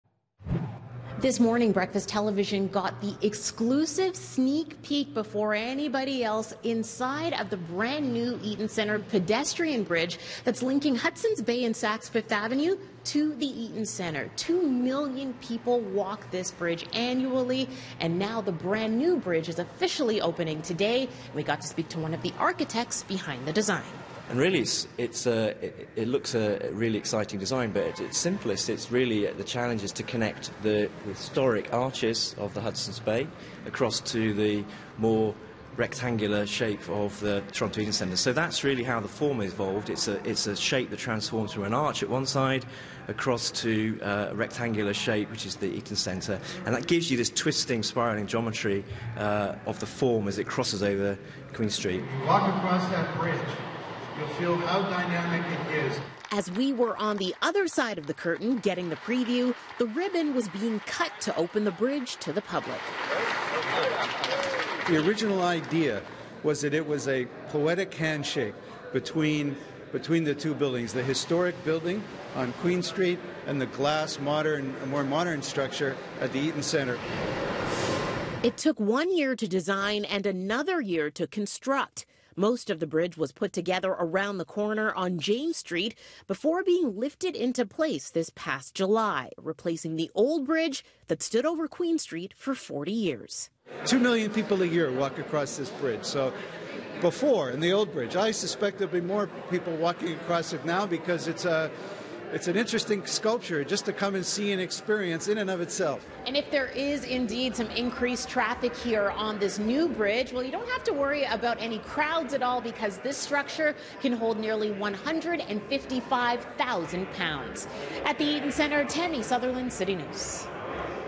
Newly-designed pedestrian bridge over Queen St. officially opened
The newly-designed pedestrian bridge over Queen Street that connects the Eaton Centre to the Hudson's Bay building was officially opened Thursday morning. Tammie Sutherland with the how the design is a "poetic handshake" between old and new.